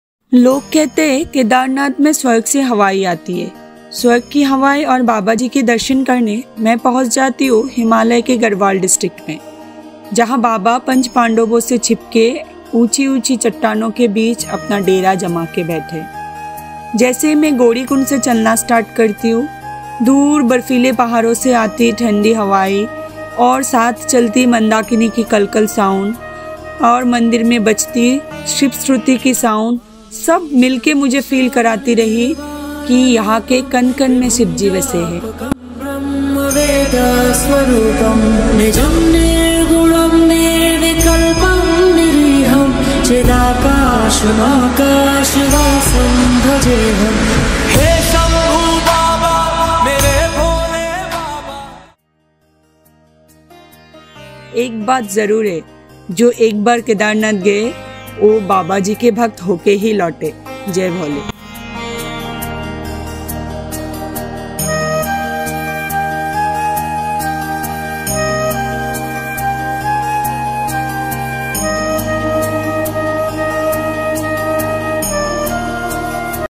kedarnath- a celestial experience through my lens|| an unforgettable solo journey#kedarnath
kedarnath is not a journey its a realisation of being close to a positive superpower ( can be called as Mahadev), experiencing the purity of nature. The whole trek was just  like meditation for me- with the sound of speedy mandakini as background music.
July 2022, one day I booked my ticket and boarded on train for uttarkhand without any fixed destination. Probably it was Mahadev's call for me. After reaching haridwar i booked my seat for kedarnath and there the journey starts...